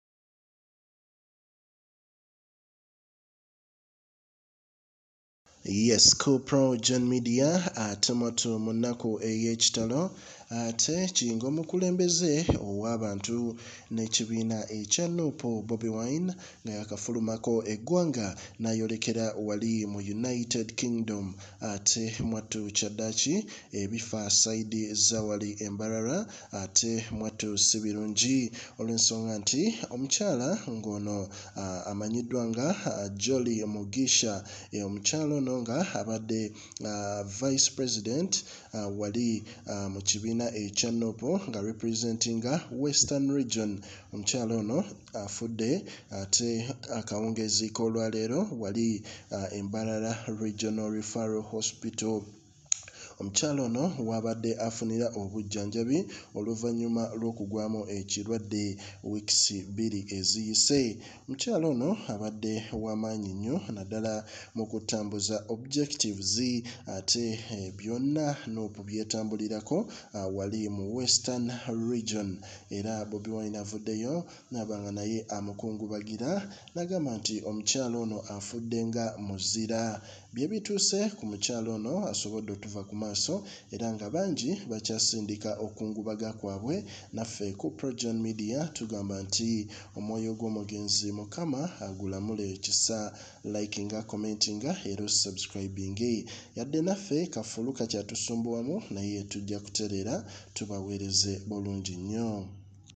Afudde Bobi wine Bambi mu UK amawulire gokufa kwa molly mugisha vice president wa nup jehamusanze
NUP Vice President for Western Region, Jolly Mugisha, Passes Away
Mbarara, Uganda

In a deeply sorrowful development, the National Unity Platform (NUP) has lost one of its key leaders, Ms. Jolly Mugisha, the party’s Vice President for the Western region. Ms. Mugisha passed away this afternoon at Mbarara Regional Referral Hospital, where she had been receiving treatment for an illness over the past two weeks.
Ms. Mugisha was a pivotal figure in the NUP, playing an instrumental role in advancing the party’s objectives and representing the Western region’s interests. Her dedication to political activism and her community endeared her to many, making her a respected and admired leader.

Over the last two weeks, Ms. Mugisha had been battling a severe illness, which led to her hospitalization at Mbarara Regional Referral Hospital. Despite receiving medical care, her condition deteriorated, leading to her untimely demise today.